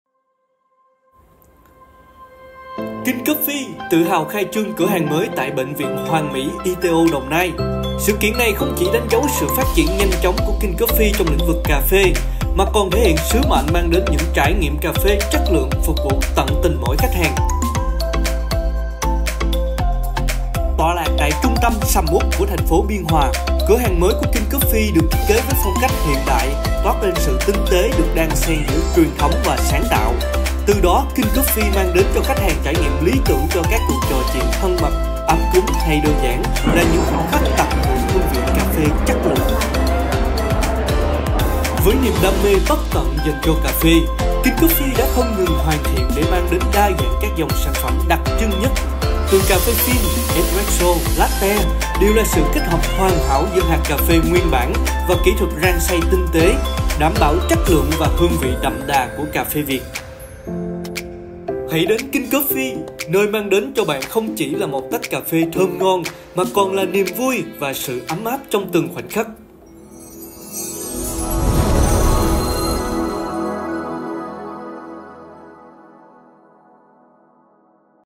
KING COFFEE TIẾP TỤC KHAI TRƯƠNG CỬA HÀNG THỨ 2 TẠI HỆ THỐNG BỆNH VIỆN HOÀN MỸ TRÊN TOÀN QUỐC ️
👏 Hân hoan chúc mừng khai trương cửa hàng King Coffee Thuận Mỹ ITO - thuộc dự án của hệ thống bệnh viện Hoàn Mỹ.  Tọa lạc tại F99 Võ Thị Sáu, Thống Nhất, Biên Hòa, Đồng Nai.

🌟 Chứng tỏ lợi thế cạnh tranh vượt trội của King Coffee trong ngành cà phê, đây được xem là cửa hàng thứ 2 của King Coffee thông qua sự hợp tác bền vững với mục tiêu mở rộng mạng lưới cửa hàng phủ khắp hệ thống bệnh viện Hoàn Mỹ trên toàn quốc.

💝 Từ đó, King Coffee mong muốn mang đến cho khách hàng cơ hội được trải nghiệm sản phẩm cà phê chất lượng, cùng với dịch vụ tận tâm.

👉 Trước đó, vào tháng 05/2024, cửa hàng của King Coffee tại Bệnh viện Hoàn Mỹ Thủ Đức (số 241 Quốc lộ 1K, Linh Xuân, Thủ Đức, TPHCM) đã chính thức đi vào hoạt động.

💓 Hướng đến phục vụ cộng đồng và đáp ứng nhu cầu ngày càng cao của khách hàng, King Coffee khẳng định sẽ còn tiếp tục là đơn vị đầu tiên và duy nhất đồng hành cùng hệ thống bệnh viện Hoàn Mỹ để mở rộng mạng lưới cửa hàng rộng khắp chuỗi bệnh viện trên toàn quốc trong thời gian tới.
👑 CHỌN CÀ PHÊ, CHỌN KING COFFEE 👑